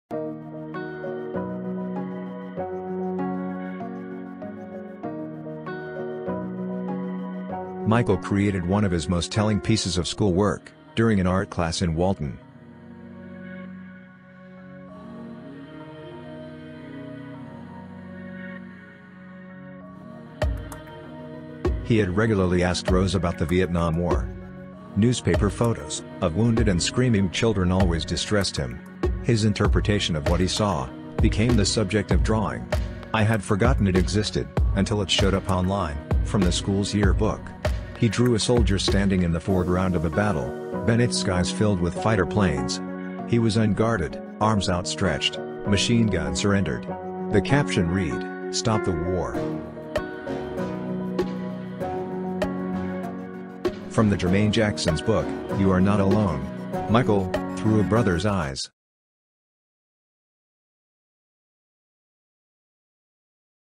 About the Michael's drawing "STOP THE WAR" from 1971
An animated film made by me talking about the Michael Jackson's drawing from 1971.

Jermaine Jackson, Michael jackson's older brother explains all in his book: You are Not Alone: Michael: Through a Brother's Eyes:

"Michael created one of his most telling pieces of school work during an artclass in Walton. He had regularly asked Rose about the Vietnam War. Newspaper photos of wounded and screamimg children always distressed him. His interpretation of what he saw became the subject of drawing. I had forgotten it existed until it showed up on-line from the school’s yearbook. He drew a soldier standing in the foregruound of a battle, benath skies filled with fighter planes. He was unguarded, arms outstretched, machinegun surrendered. The caption read, STOP THE WAR."